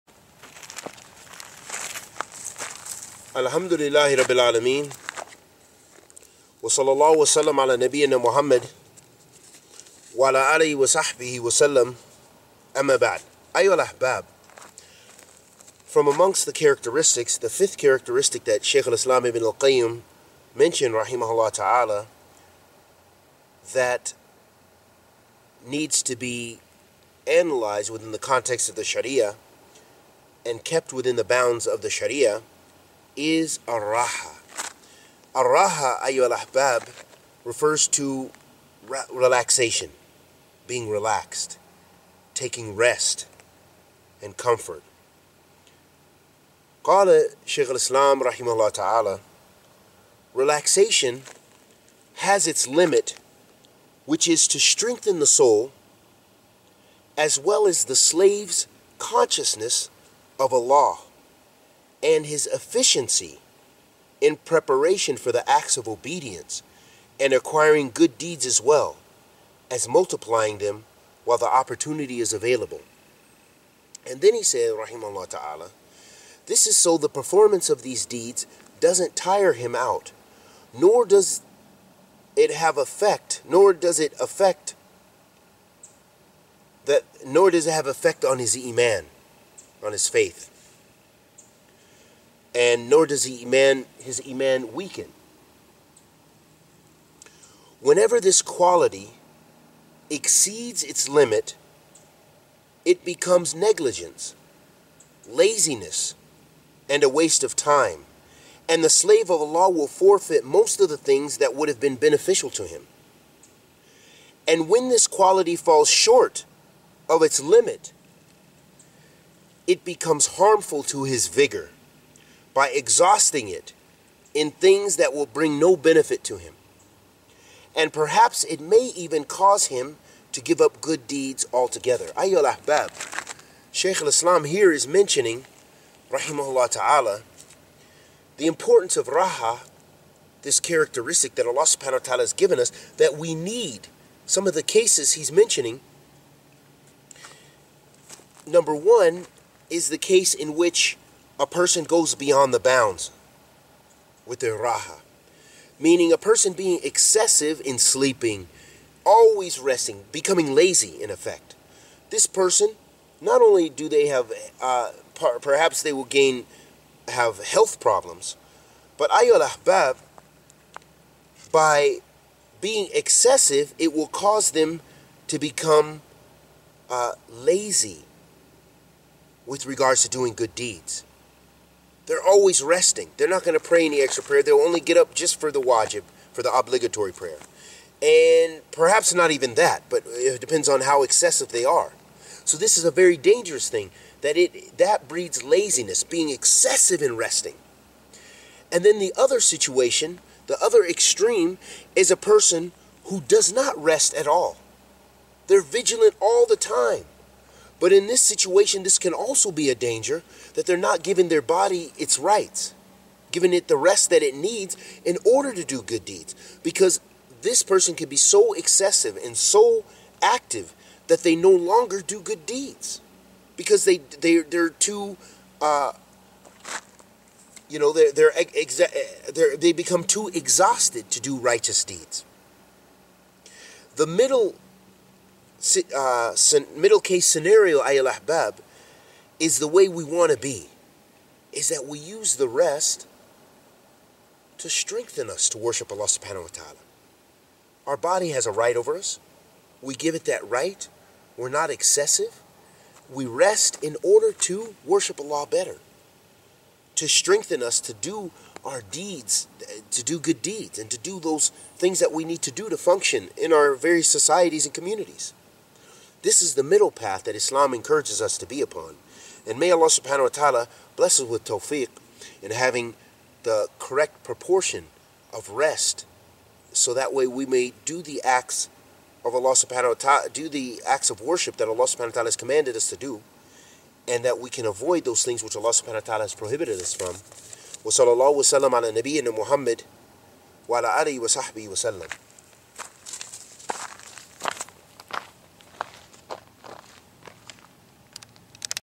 Character of The Believer: Relaxation and Rest For Worship
How the believer prepares his body for worship